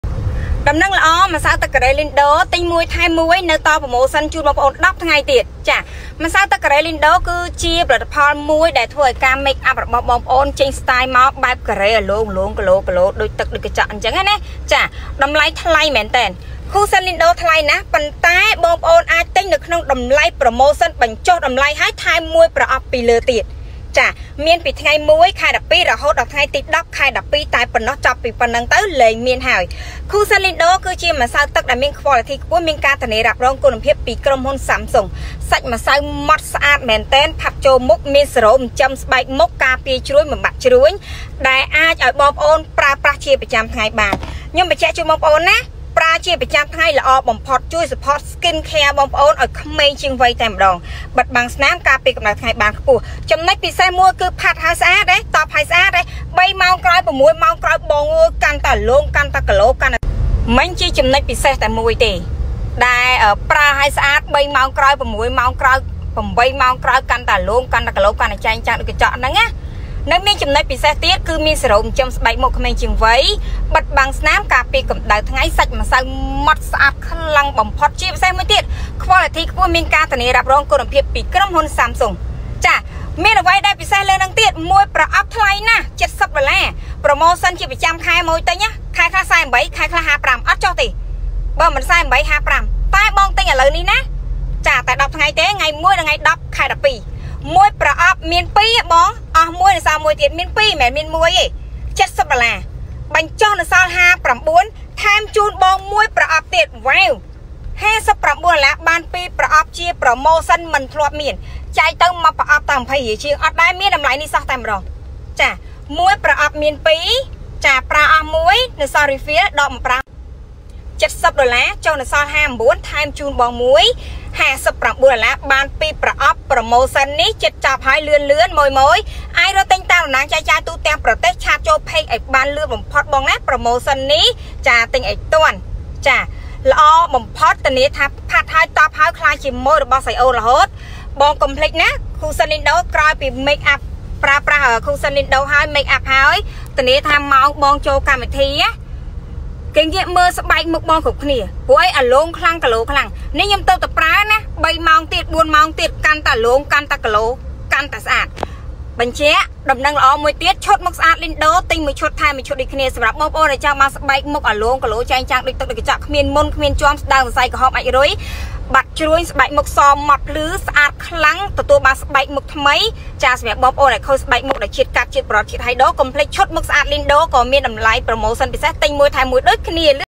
ឈុតមុខស្អាតលីនដូរបញ្ចុះតម្លៃ | Ah Nita សម្រស់ធម្មជាតិ
🔥ក្ដៅៗឈុតមុខស្អាតលីនដូរបញ្ចុះតម្លៃ ទិញថែមច្រេីនមិនធ្លាប់មាន
🌷ធានាថាប្រេីហេីយស្បែកមុខស្អាតខ្លាំងៗ ណាស់ ម៉្សៅទឹកកូរ៉េមានសេរ៉ូមចិញ្ចឹមស្បែកមុខប្រេីរាល់ថ្ងៃកាន់តែក្មេងវ័យ សាច់ម៉្សៅហ្មត់ស្អាត ផាត់ចូលមុខល្អ ទុកកាន់តែយូរម៉ោងកាន់តែរលោង ភ្លឺចែងចាំងដូចកញ្ចក់
🌷បិតបាំងស្នាមបានល្អ ការពារកំដៅថ្ងៃបានខ្ពស់ ដំណឹងសម្រាប់ម៉ូយ
🌷 1ប្រអប់ មាន2ខ្លង 70$ លក់ជូន 2ប្រអប់ តែ 59$ ទេ
🌷 លក់តែ 10 ថ្ងៃទេ ថ្ងៃ 01 ដល់ 10 ខែ 12 ប៉ុណ្នោះ

                                                      សូមអរគុណ...